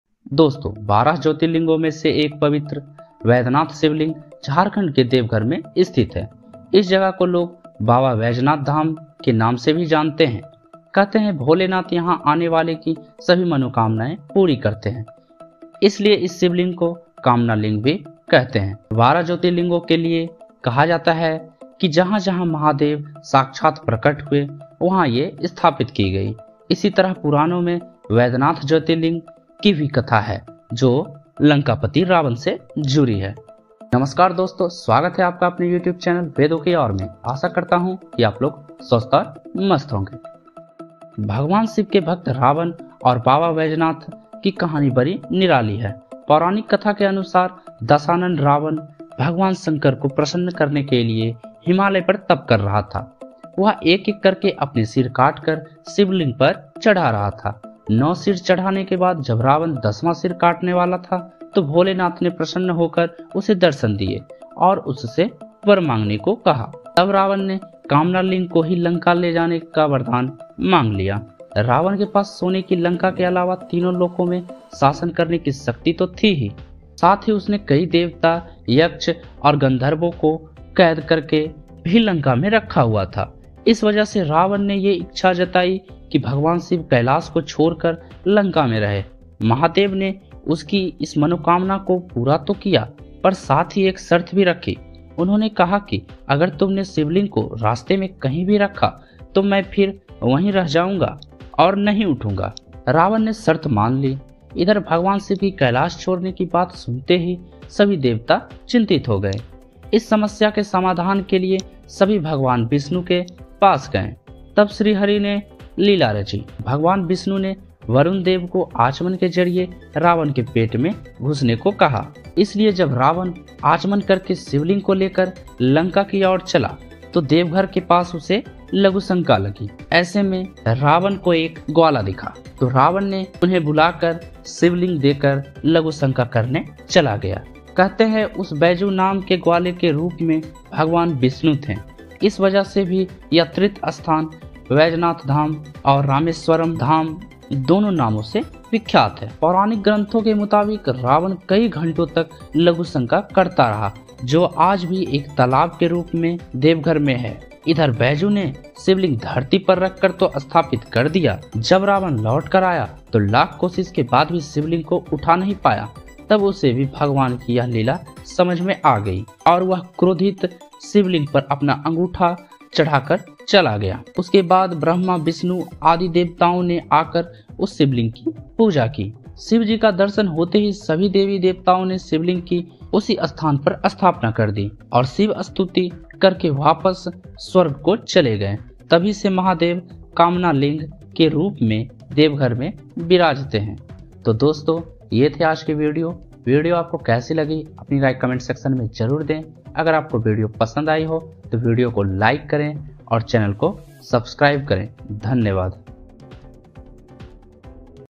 बाबा बैजनाथ धाम की कथा |Baba baidyanath dham
Baba baidyanath dham
 धर्म में बारह ज्योतिर्लिंगों के दर्शन का बड़ा महत्व है. इन सभी से शिव की रोचक कथाएं जुड़ी हुई हैं. देवघर के वैद्यनाथ धाम में स्थापित 'कामना लिंग' भी रावण की भक्ति का प्रतीक है.
बाबा बैजनाथ धाम की कथा:
भगवान शिव के भक्त रावण और बाबा बैजनाथ की कहानी बड़ी निराली है. पौराणिक कथा के अनुसार दशानन रावण भगवान शंकर को प्रसन्न करने के लिए हिमालय पर तप कर रहा था. वह एक-एक करके अपने सिर काटकर शिवलिंग पर चढ़ा रहा था. 9 सिर चढ़ाने के बाद जब रावण 10वां सिर काटने वाला था तो भोलेनाथ ने प्रसन्न होकर उसे दर्शन दिए और उससे वर मांगने को कहा.

तब रावण ने 'कामना लिंग' को ही लंका ले जाने का वरदान मांग लिया. रावण के पास सोने की लंका के अलावा तीनों लोकों में शासन करने की शक्ति तो थी ही साथ ही उसने कई देवता, यक्ष और गंधर्वो को कैद कर के भी लंका में रखा हुआ था. इस वजह से रावण ने ये इच्छा जताई कि भगवान शिव कैलाश को छोड़ लंका में रहें. महादेव ने उसकी इस मनोकामना को पूरा तो किया पर साथ ही एक शर्त भी रखी. उन्होंने कहा कि अगर तुमने शिवलिंग को रास्ते में कही भी रखा तो मैं फिर वहीं रह जाऊंगा और नहीं उठूंगा. रावण ने शर्त मान ली.